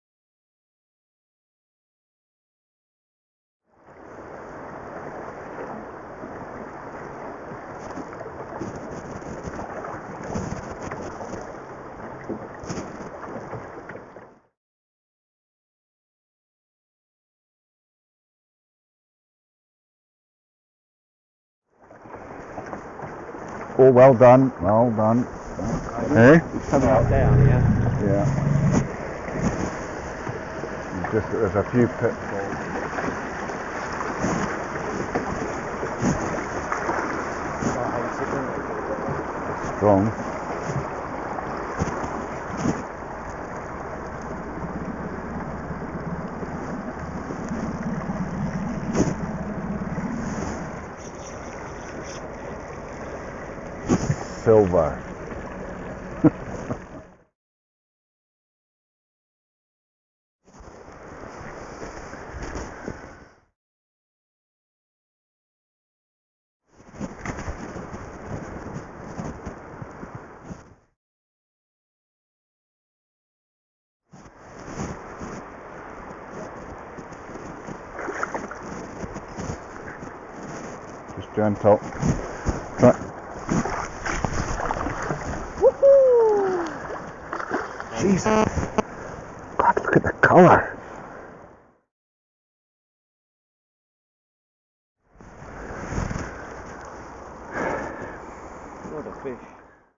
Salmon Fishing Scotland Spring Salmon fly fishing on the Tay at Stanley, Perthshire February 2016.
A spring salmon being hooked, played, landed and returned on the Tay at Stanley, Perthshire in February 2016 by Peter Fine.